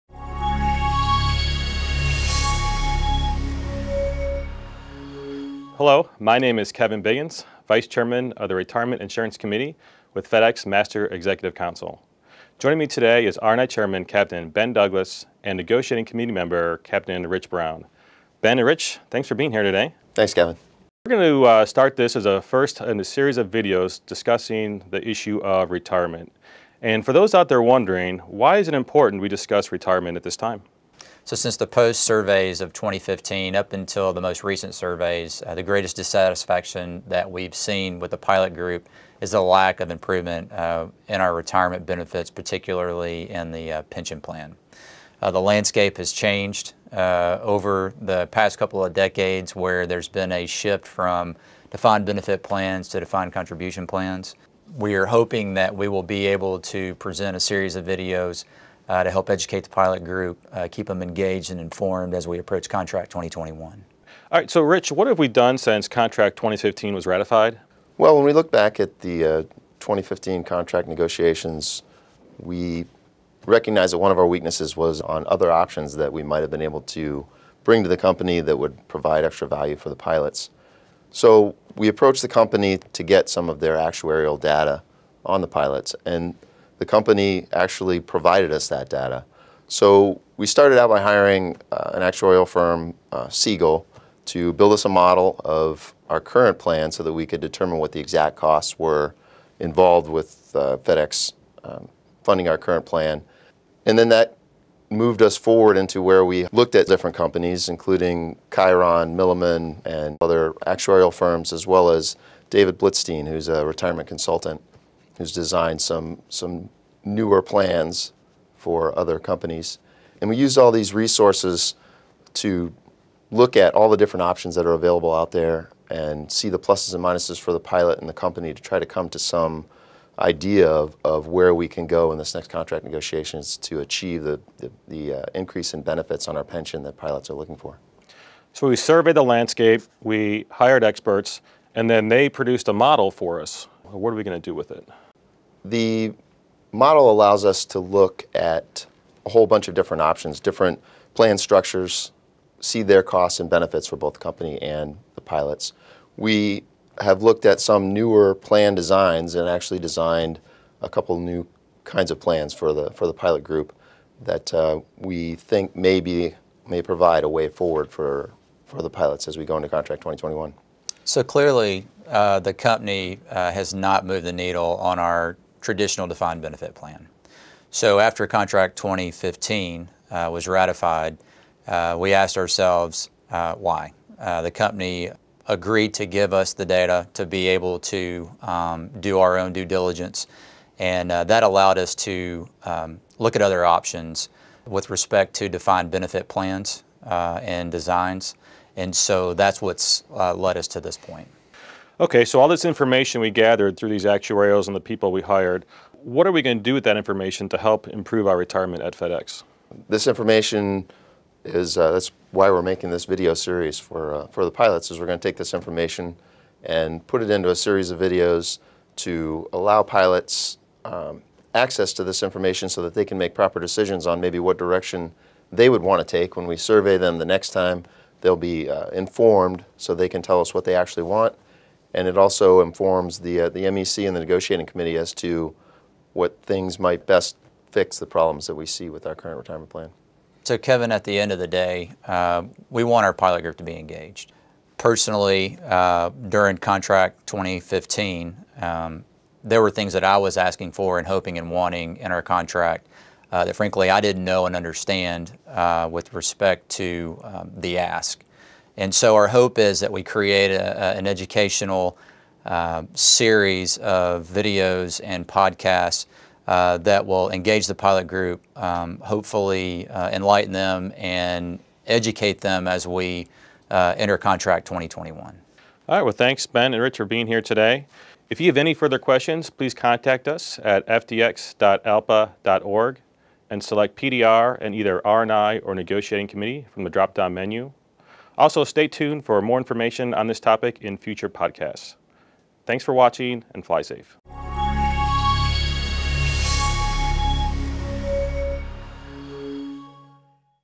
Retirement Education Series Introduction (Video 1)
In this video, Captain Ben Douglas (Retirement & Insurance Chairman), Captain Kevin Biggins (Retirement & Insurance Vice Chairman), and Captain Rich Brown (Negotiating Committee Member) discuss the upcoming Retirement Education Series.